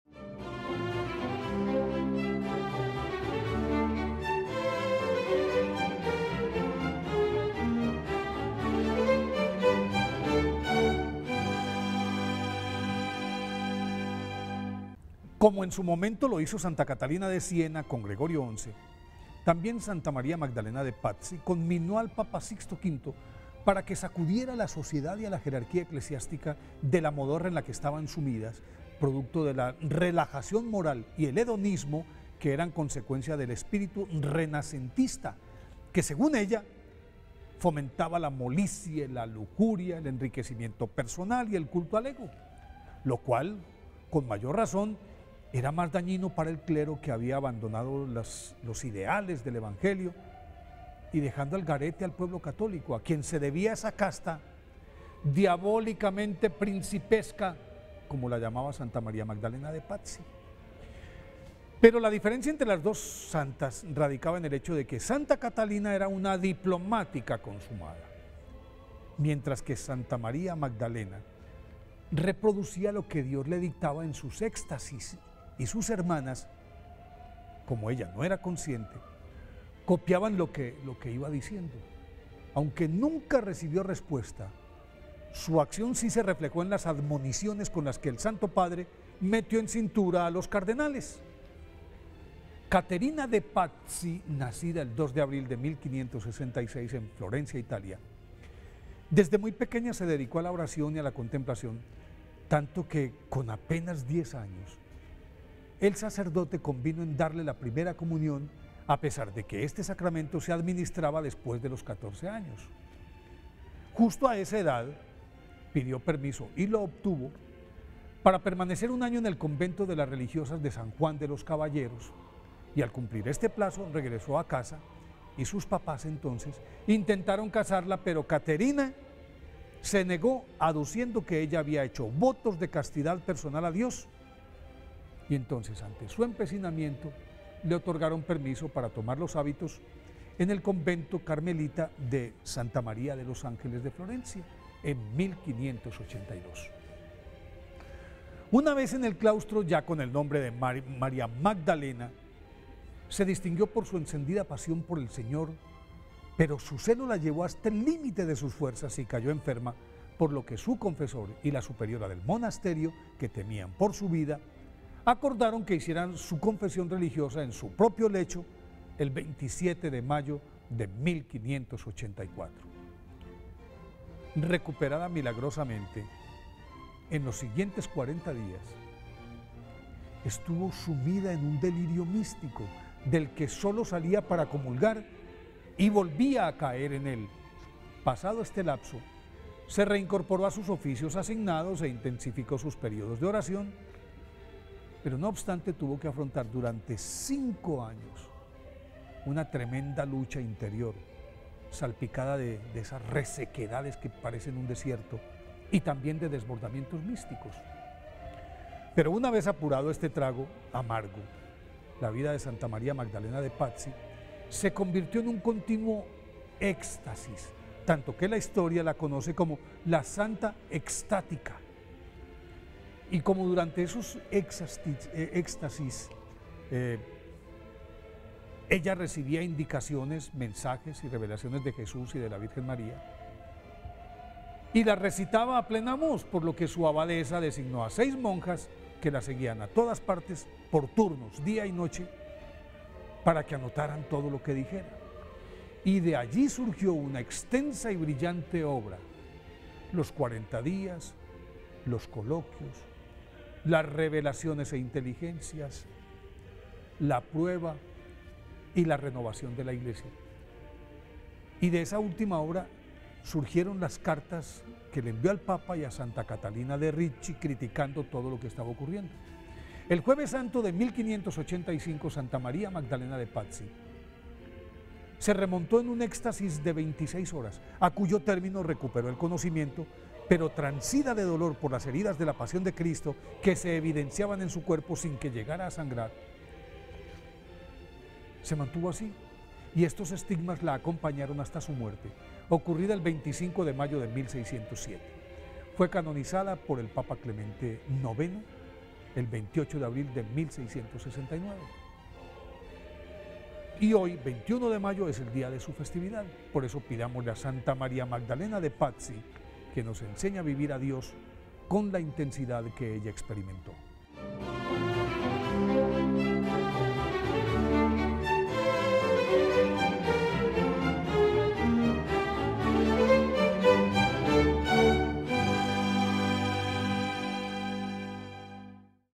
21 de mayo día de Santa María Magdalena Pazzi, Santo del Día - Tele VID
Conoce un poco de la historia de cada uno de los santos en su día. Hoy 21 de mayo celebramos el día de Santa María Magdalena Pazzi.
Ingresa y conoce la historia de cada Santo.